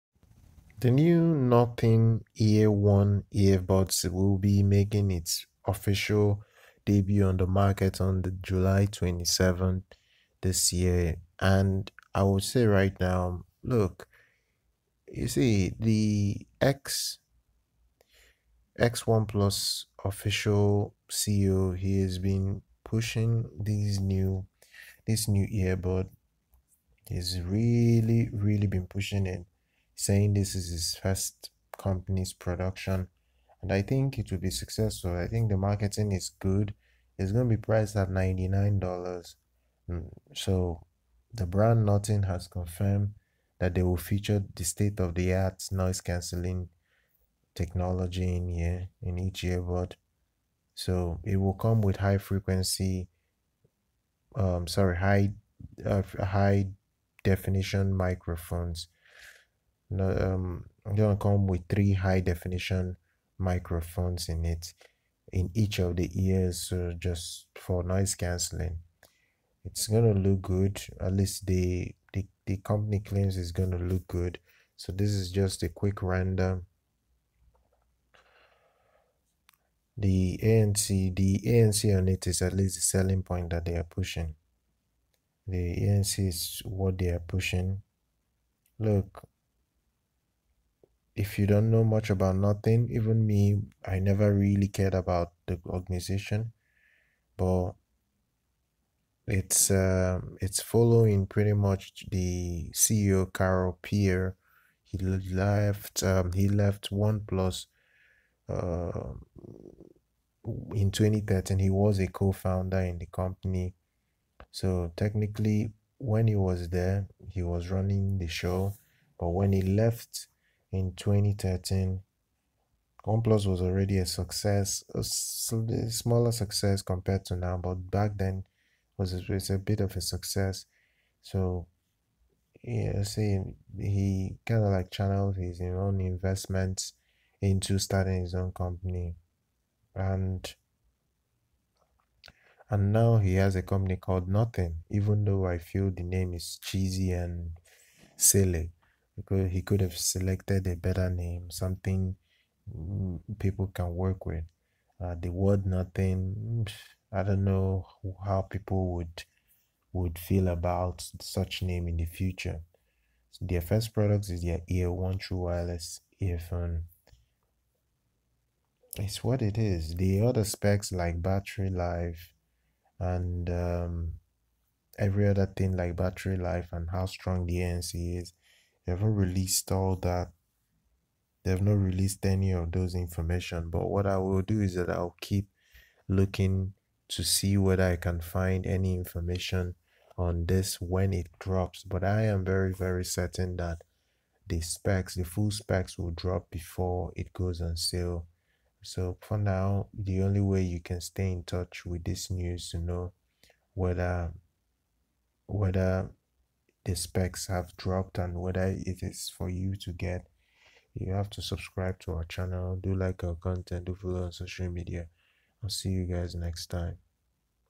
Nothing Ear 1 earbuds will debut at $99 and have noise canceling upon it's July 27 debut
The Nothing Ear 1 true wireless earbuds will launch at 2 p.m. BST on July 27 in the U.K., which is 6 a.m. PT and 9 a.m. ET in the U.S. They will cost $99 USD and though much still remains unknown about their design, Nothing has confirmed that they will feature “state-of-the-art” active noise cancellation (ANC) and each earbud will sport three high-definition microphones.If you’re new to Nothing, here’s why you should be paying attention. It’s the new company created by Carl Pei following his departure from OnePlus, which he co-founded with Pete Lau in 2013. Given OnePlus’ success, the phones it has produced, and the strong fan base it has built, there is plenty of anticipation surrounding Pei’s plans for Nothing. What we know so far is that Nothing will produce “smart devices,” with the first being the Ear 1 true wireless headphones, but we don’t know exactly what its future output will be. We don’t know much about the Ear 1 yet either.

JadeAudio(FiiO) EA1 Earphone

KZ EDX Earphone

KBEAR Expansion 24 Cores Cable

KBEAR S1 Bluetooth Earphone Cable